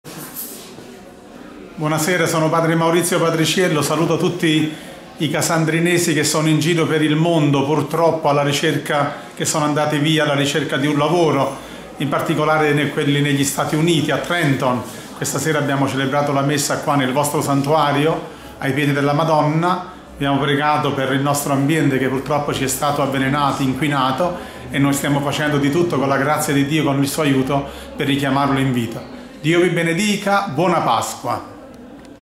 Il saluto di Don Maurizio Patriciello a tutti i Casandrinesi
28 Febbraio 2015 in occasione della celebrazione della santa messa presso il Santuario Maria SS. Assunta in Cielo (Casandrino)